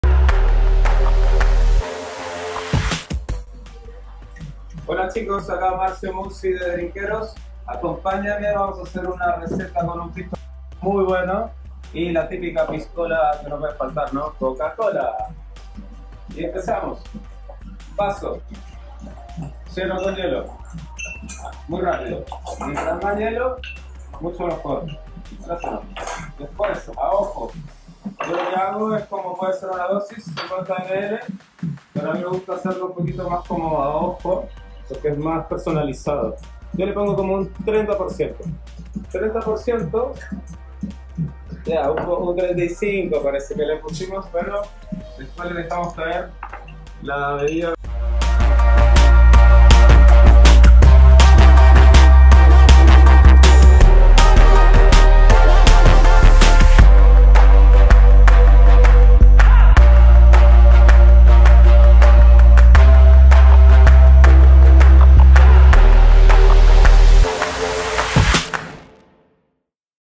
Piscola by Marzio Munzi - DrinkerosAL
Marzio nos prepara una excelente Piscola con Pisco Waqar 😎👌 hmmmmmmm